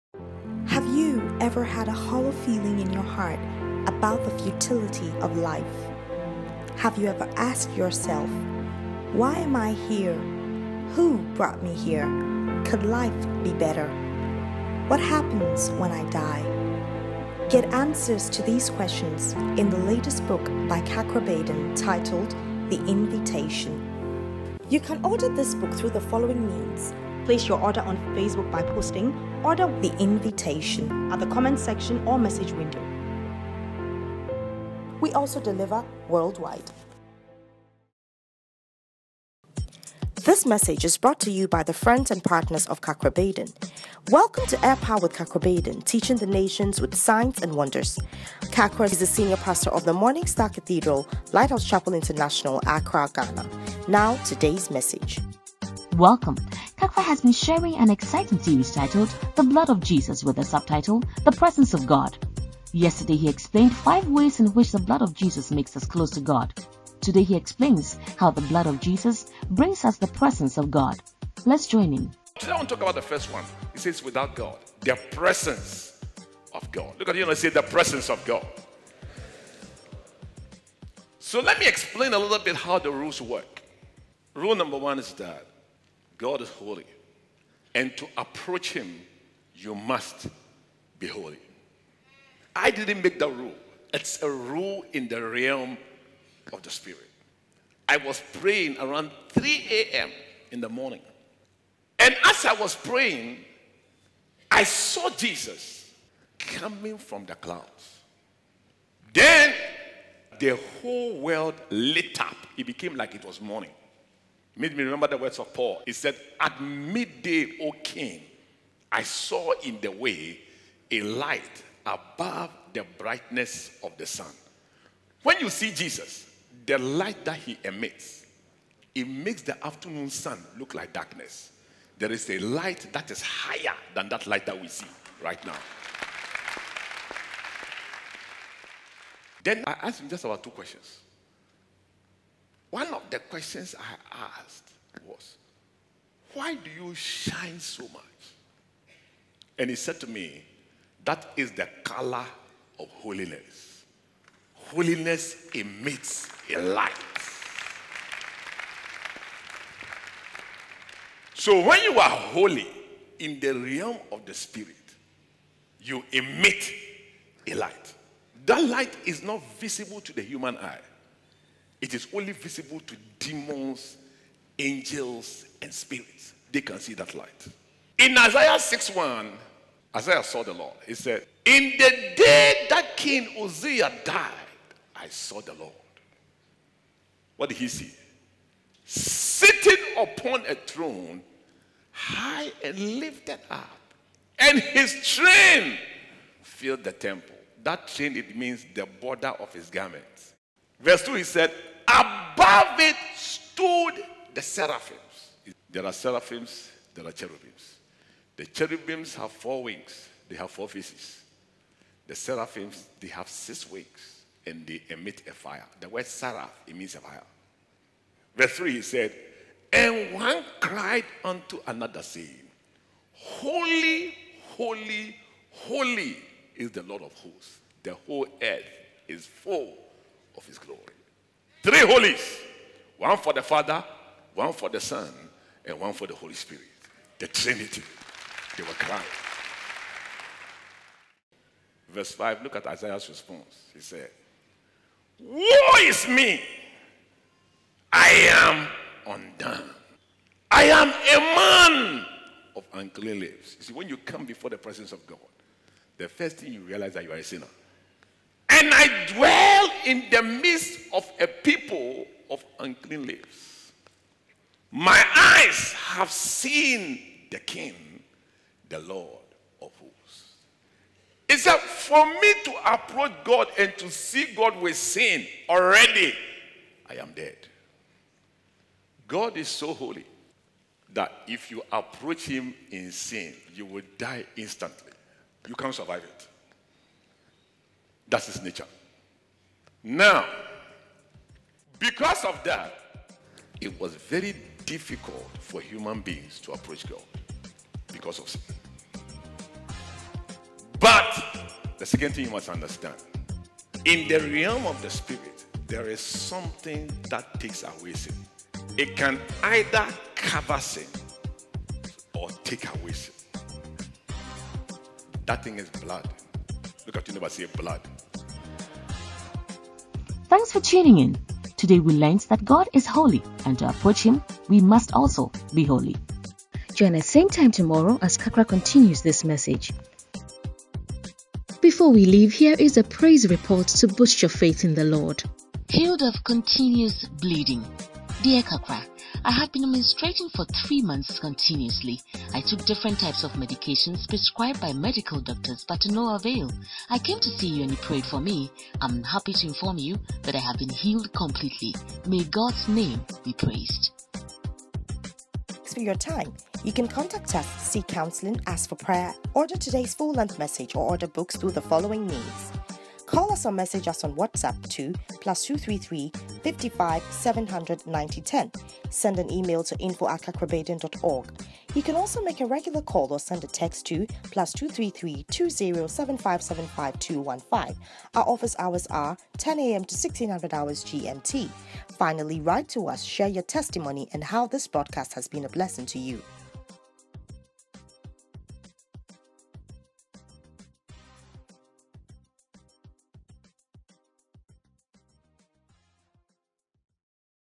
The Blood of Jesus - Holiness
Kakra sheds light on the nature of God in this powerful message. Let’s join him

Grab the full-length message by clicking here:
1) App Store:
To install.